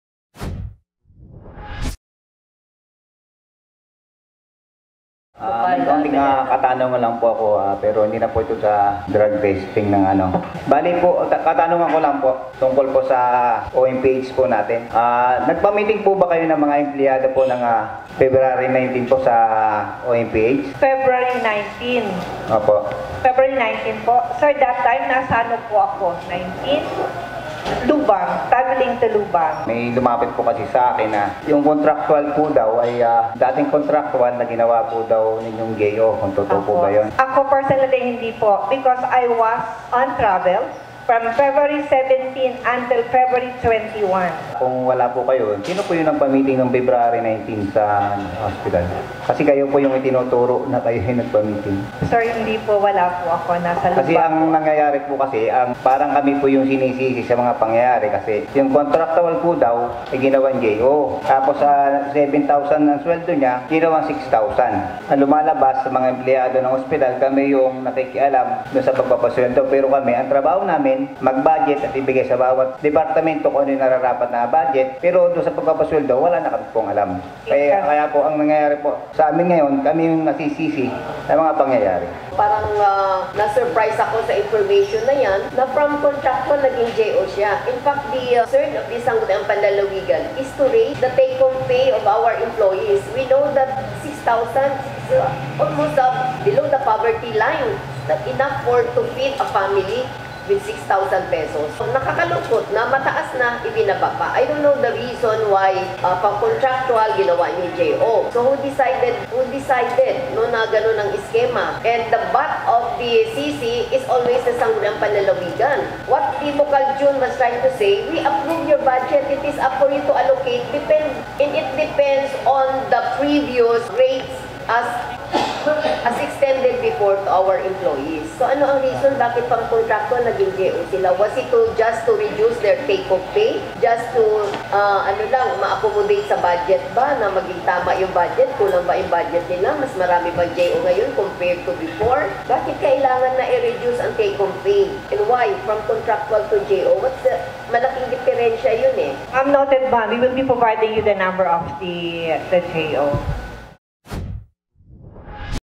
PUNTO MINDORO | BM ABELEDA AT PABLO DISMAYADO SA PAGBABAGO NG EMPLOYMENT STATUS
PUNTO MINDORO
BM ABELEDA AT PABLO DISMAYADO SA PAGBABAGO NG EMPLOYMENT STATUS
Laking gulat at pagkadismaya ang ipinahayag nina Bokal Jun Abeleda at Bokal Sonia Pablo sa balita na binago ang status ng mga empleyado kung saan mula sa pagiging contractual ay ginawang Job Order na lang sila na nagresulta din ng pagbaba ng kanilang sweldo.